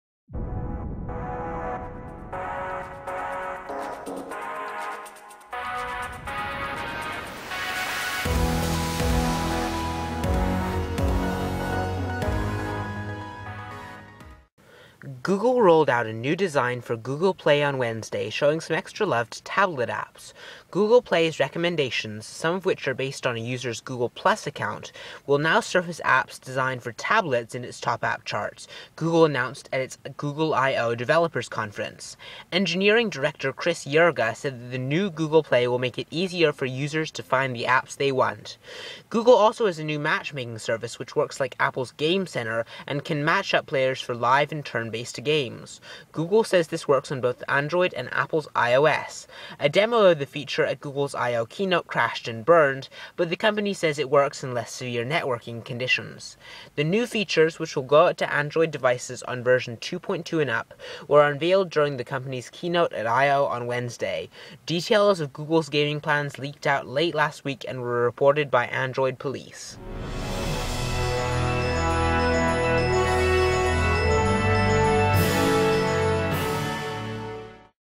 The New Redesigned Google Play
Google has redesigned their Play app store to focus on tablet apps and to make finding new apps easier.